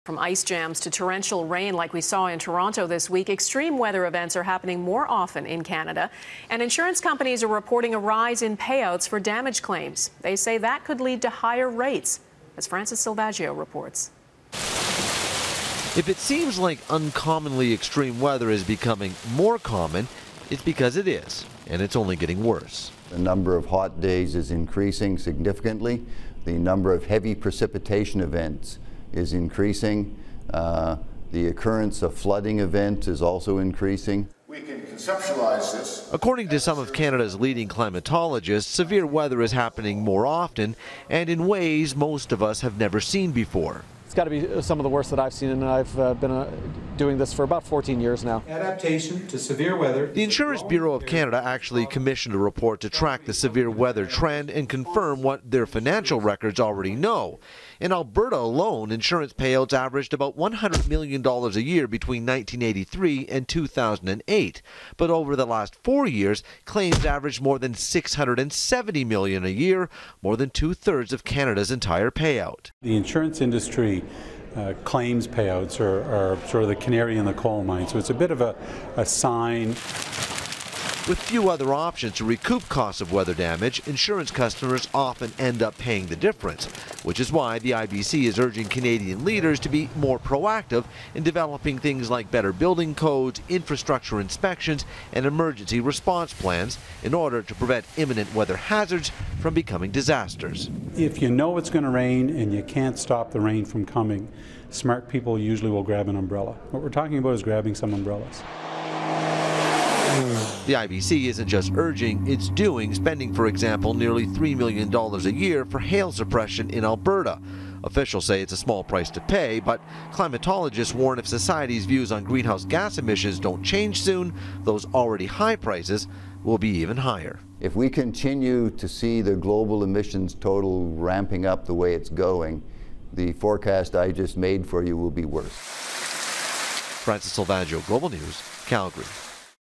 Extreme weather insurance
Thu, May 30: Insurance companies are reporting a rise in payouts for extreme weather damage claims. They say that could lead to higher rates. Francis Silvaggio reports.